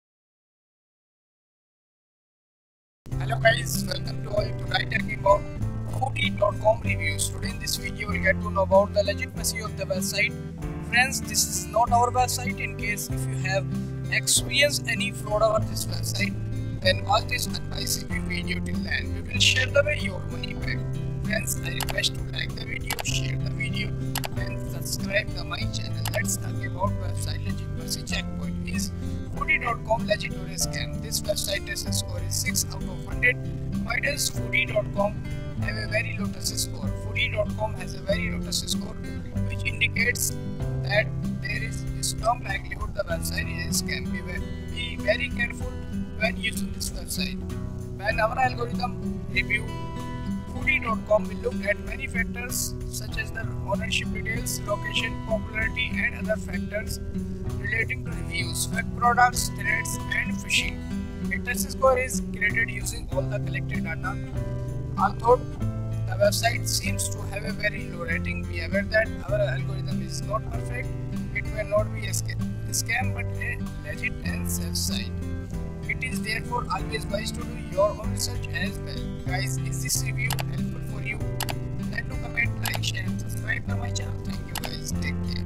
Fuuddy Reviews (April 2022) [ with 100% Proof ] SCAM or LEGIT ? 😲 Fuuddy.com Reviews
Do you want to know about Fuuddy Reviews? Watch this video till end because in this video you will get to know about the legitimacy of the website.

Product Review